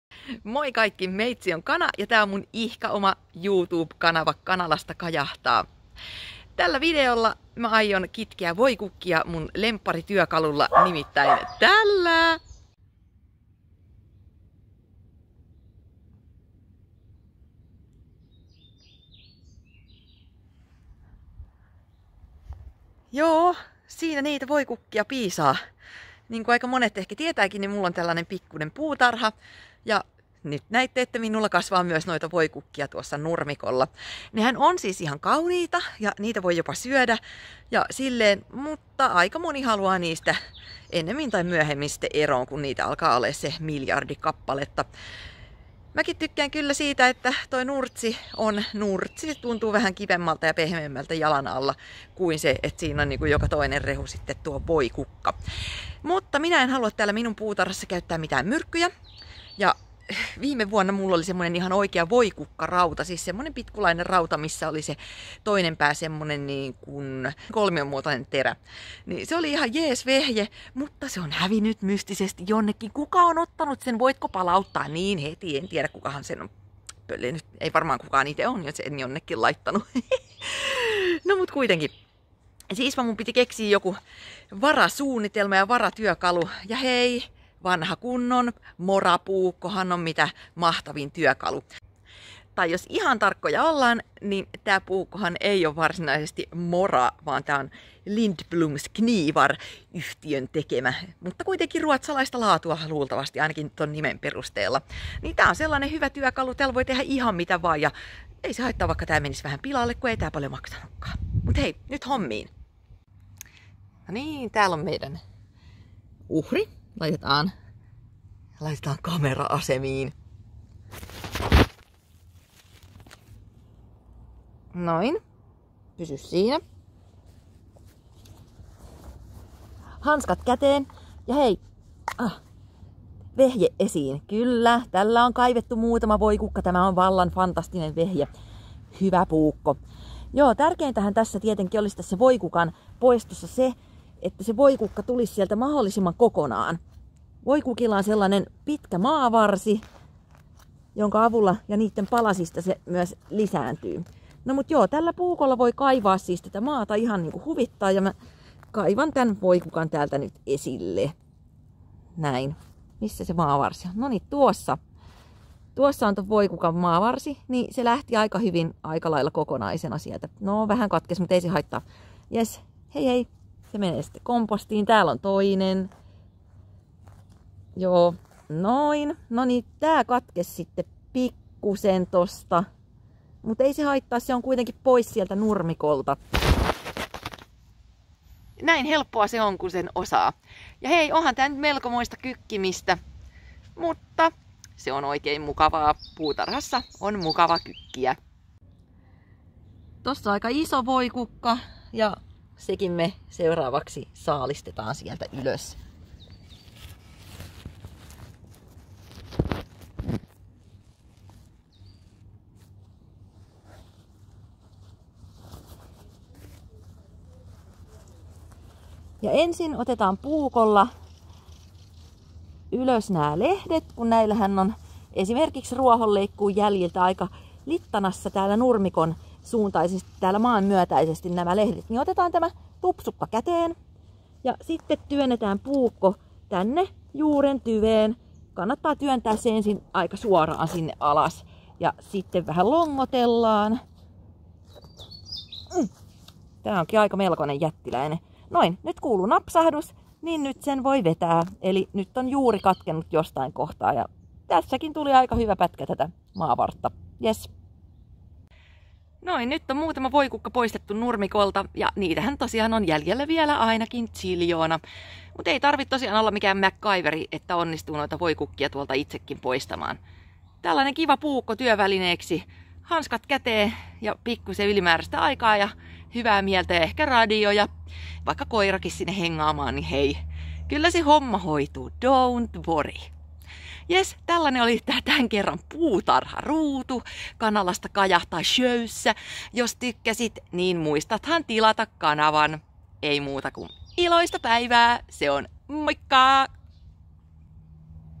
Kitken voikukkia lempityökalullani | PUUTARHA
Näin voi kitkeä nurtsista voikukkia. Teen tätä hommaa aina, kun on aikaa. Hermo lepää ja pää tyhjenee turhista hömpötyksistä. Kitkeminen on oikeesti aika terapeuttista. 😄

Mikä sun lempityökalu puutarhassa on? Heitä kommenttia.

Ja muista tilata mun kanava, jos diggasit vidistä. 😍